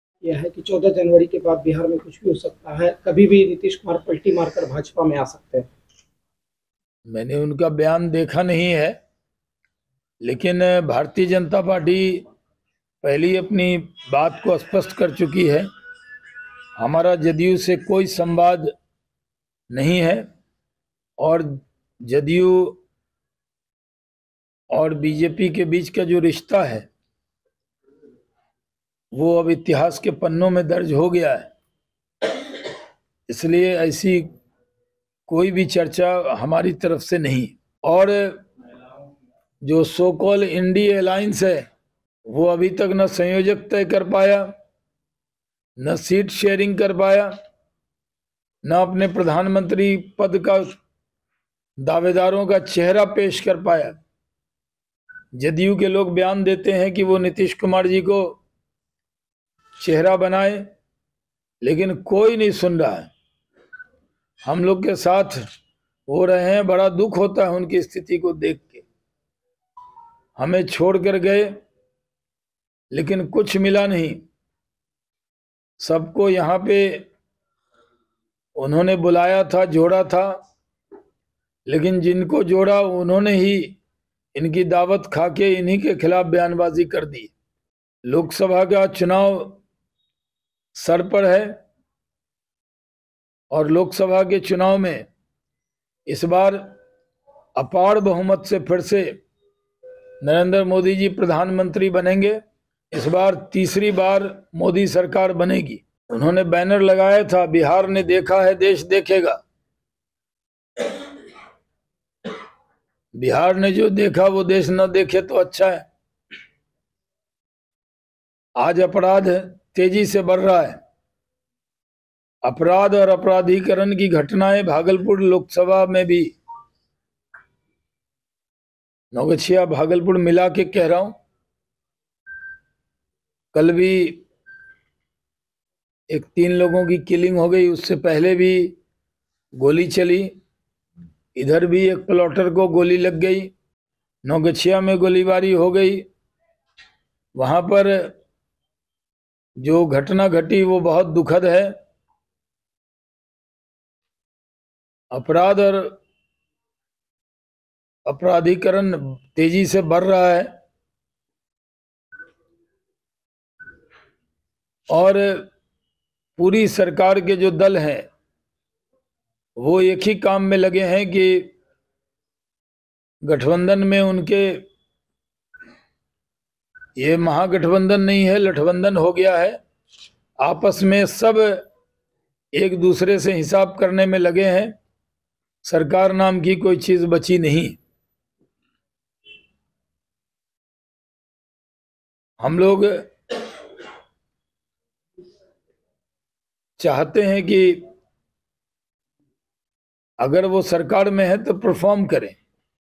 BJP नेता Shahnawaz Hussain ने JDU-BJP के रिश्तों को बता दिया इतिहास। Bihar
बिहार के पूर्व सीएम जीतनराम मांझी ने सत्ता पलटने का दावा करते हुए कहा कि खरमास के बाद नीतीश कुमार बड़ा खेला कर सकते हैं.. इस बयान के बाद बीजेपी के राष्ट्रीय प्रवक्ता शाहनवाज हुसैन ने कहा कि बीजेपी ने पहले ही ये स्पष्ट कर दिया है कि जेडीयू से बीजेपी का कोई संवाद नहीं है..